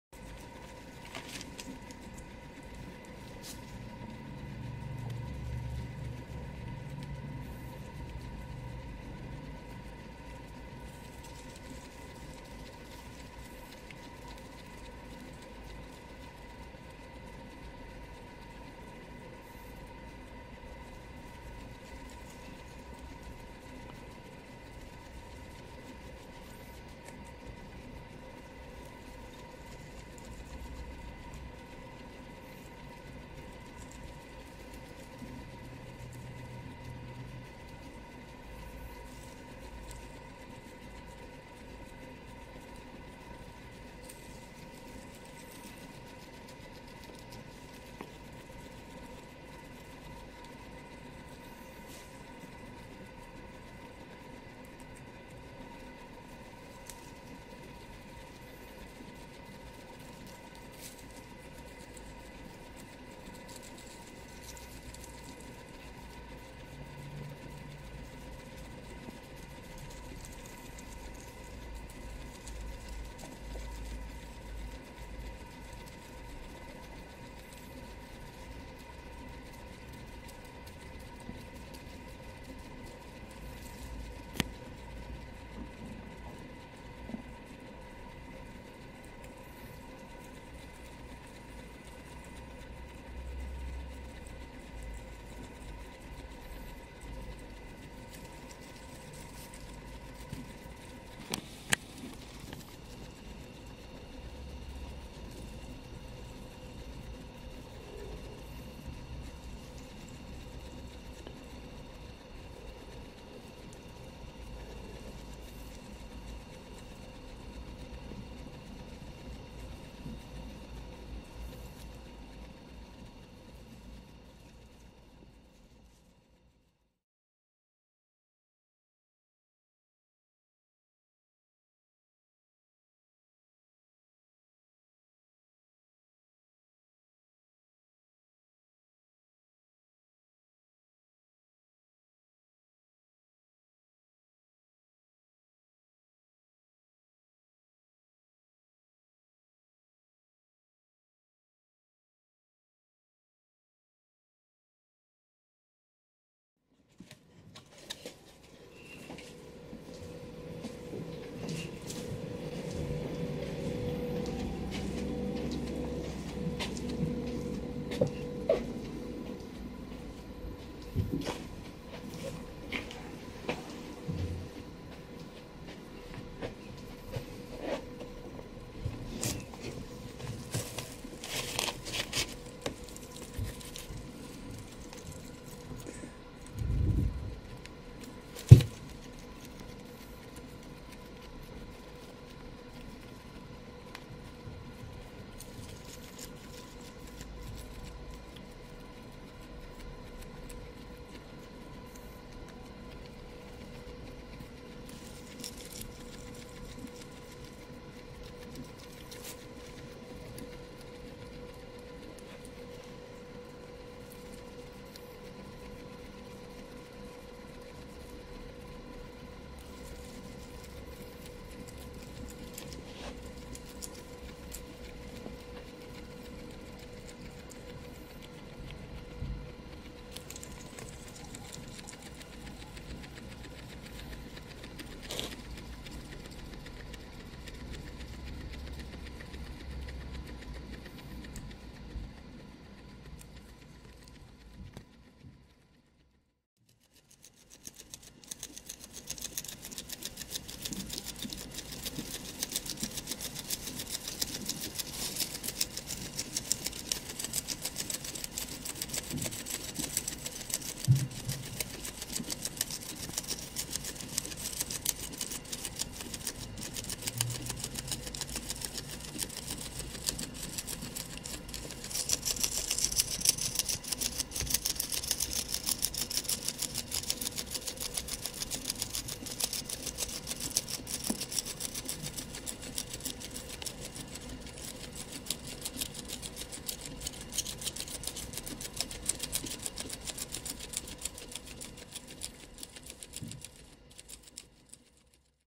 Elsa & Percy 21 & 22 i 2022
Lunch for the guinea pigs, Percy and Elsa, on Friday and Saturday, 21 & 22 January 2022.